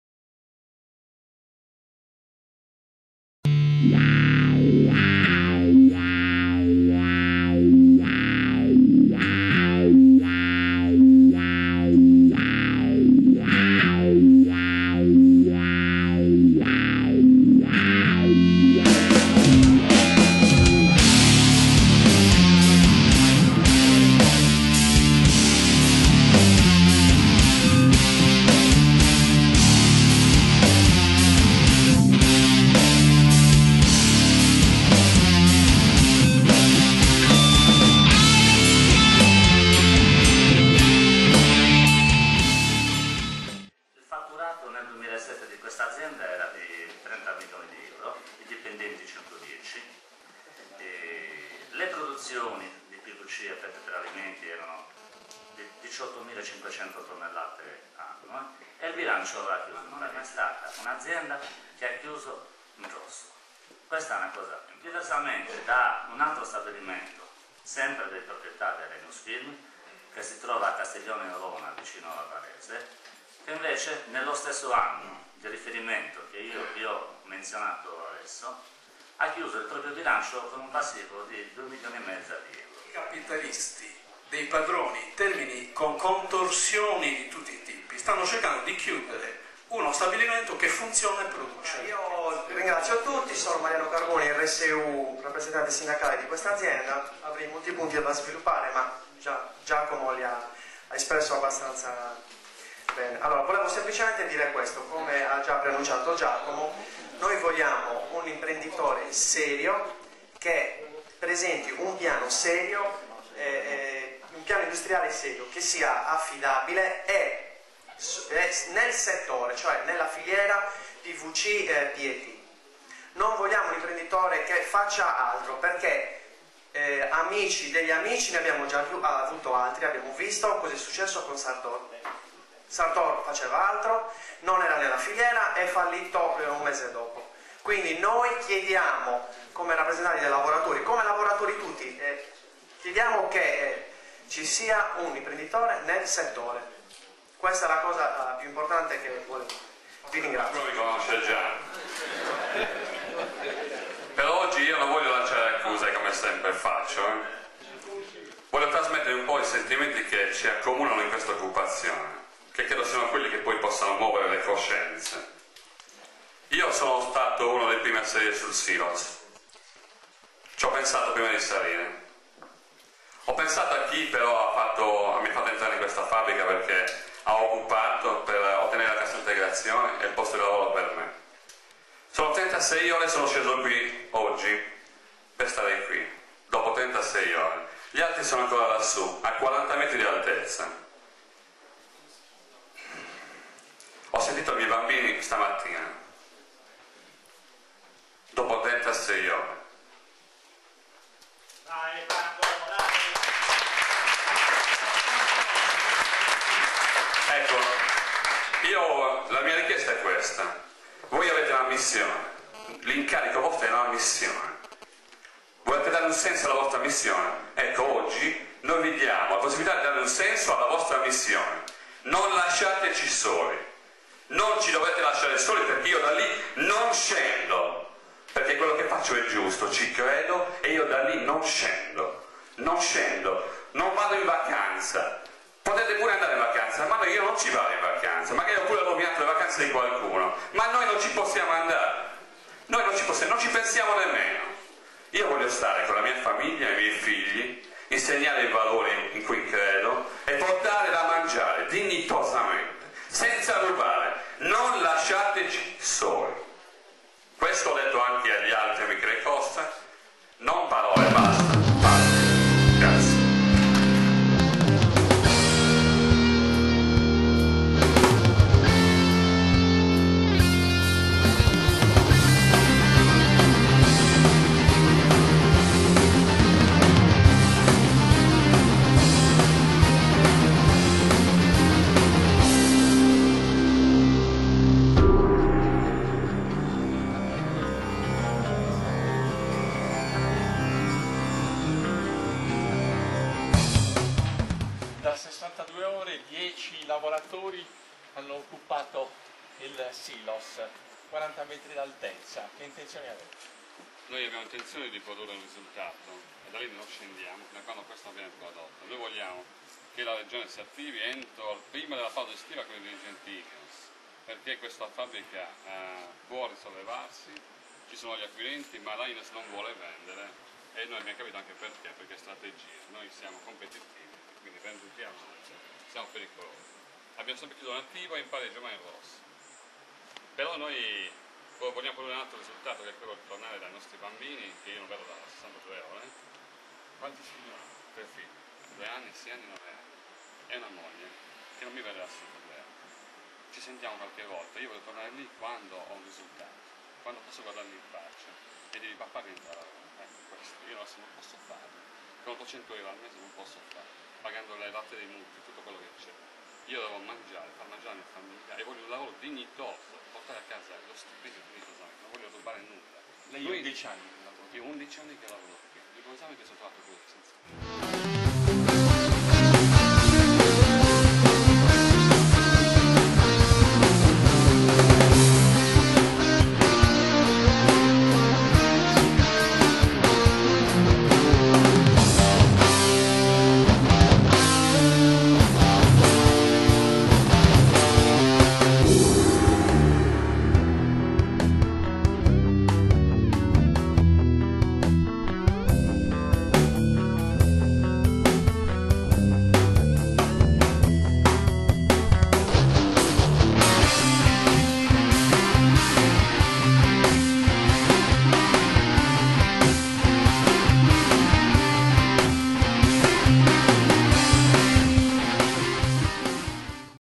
Okkupazione Ineos Films Assemini
I lavoratori dello stabilimento Ineos Films di Assemini decidono di occupare lo stabilimento dopo che per l' ennesima volta l' azienda non ha dato risposte sul futuro dei 101 posti di lavoro degli operai.